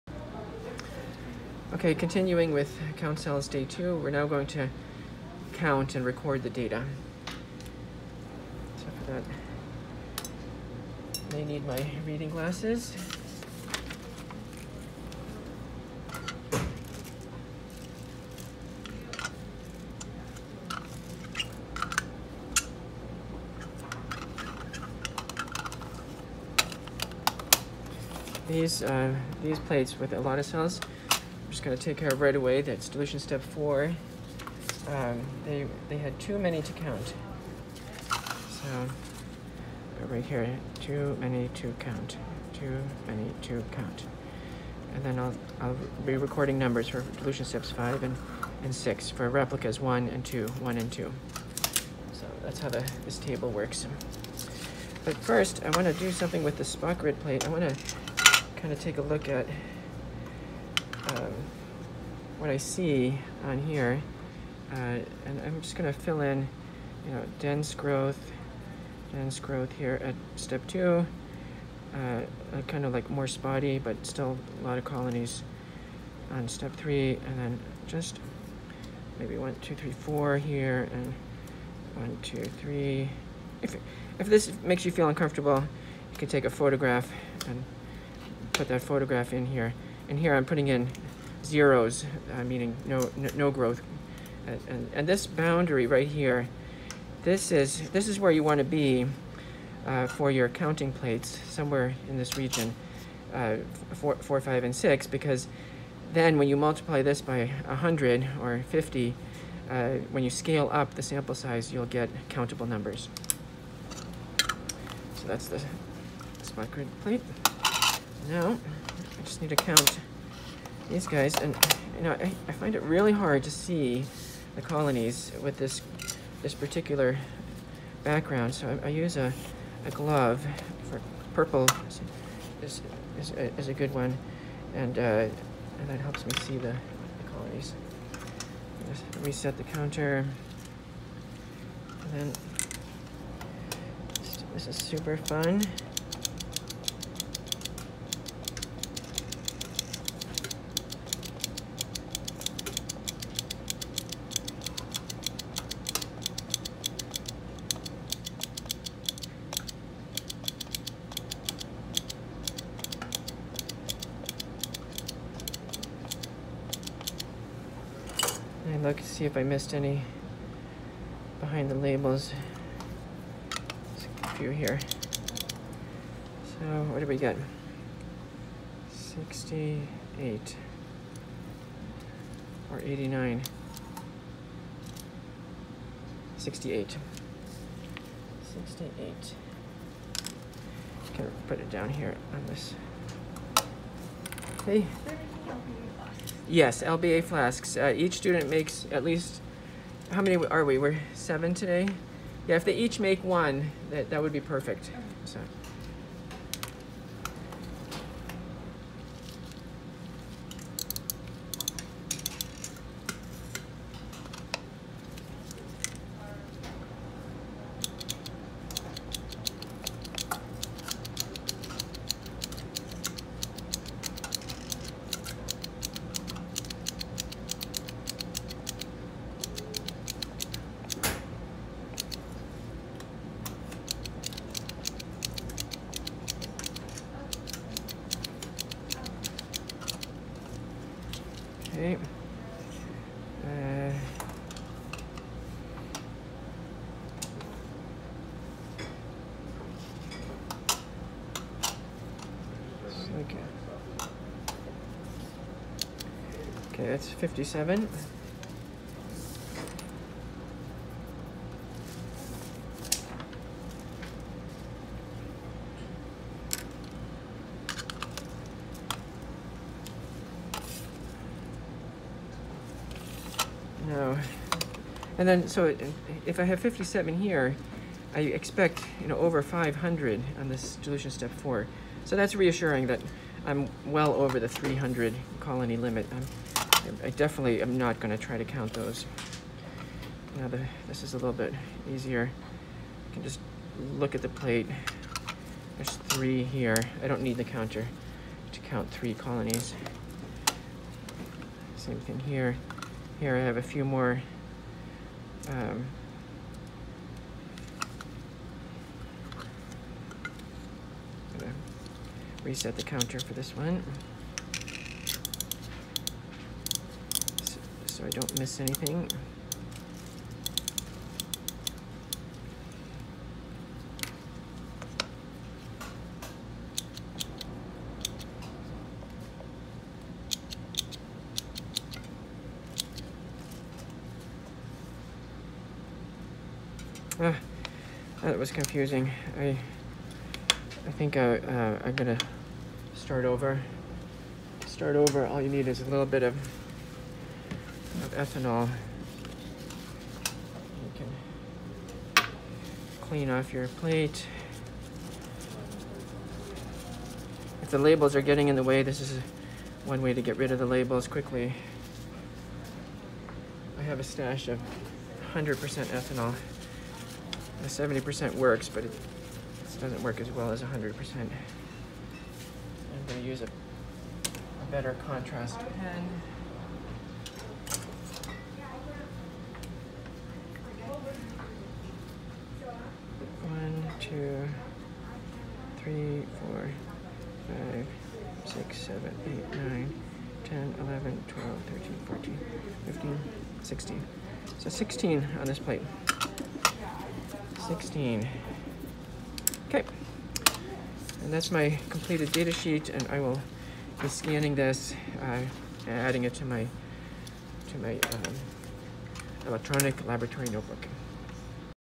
Count Cells Day 2 data collection
Instructor demonstrates the counting cells by counting colonies process